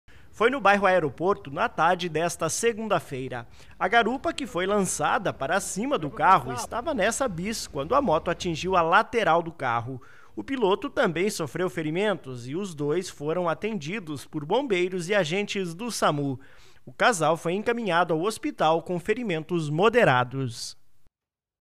Garupa de moto vai parar em cima de carro após acidente
Acidente: O choque foi em Francisco Beltrão na tarde da segunda-feira (27).